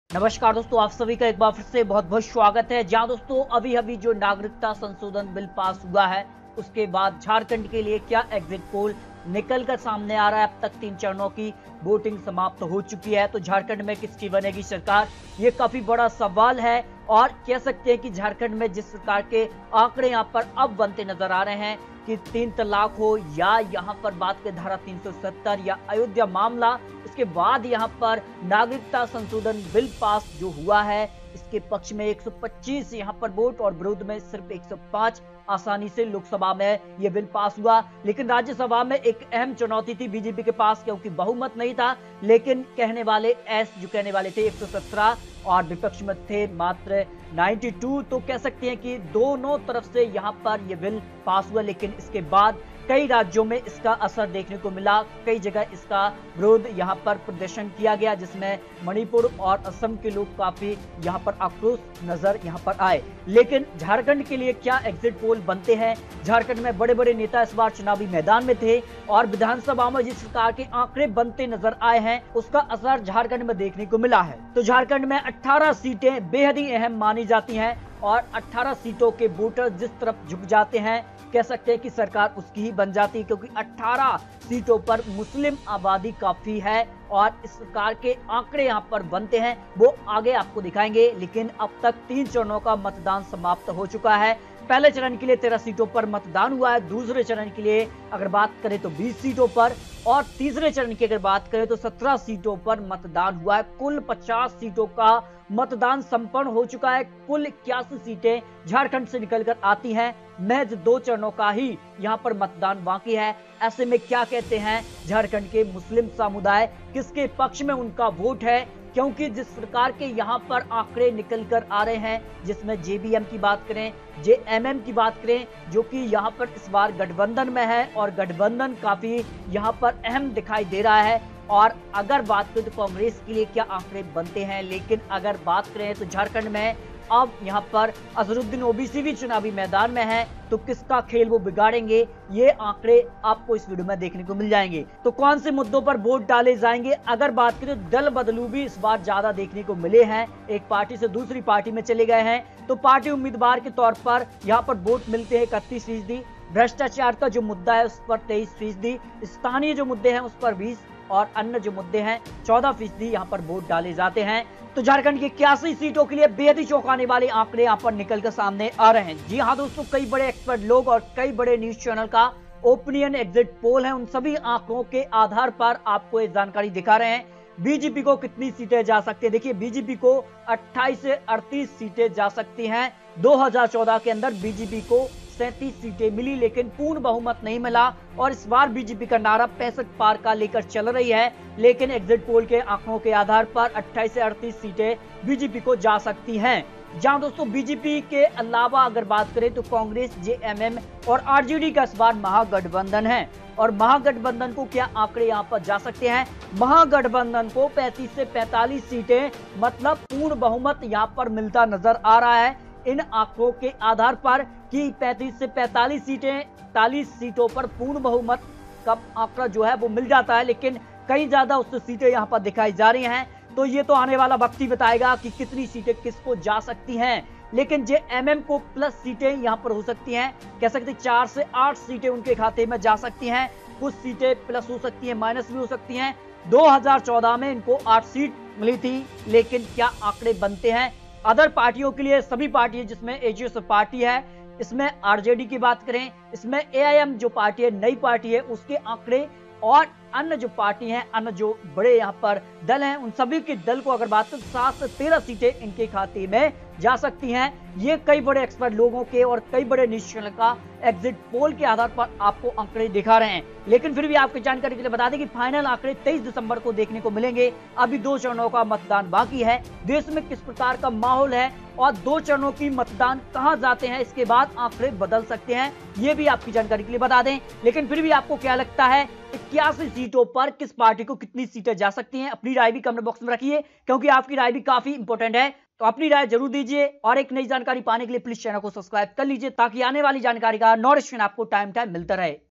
नागरिकता संशोधन बिल पास होने के बाद ? झारखंड-Exit Poll 3 चरणों के बाद कौन बनेगा cm? ताजा आंकड़े
नागरिकता संशोधन बिल पास  होने के बाद ?
झारखंड-Exit Poll 3 चरणों के बाद कौन बनेगा cm?
 ताजा आंकड़े Breaking news today
kishan news
news 2019
new vacancy
bharti police
ipl news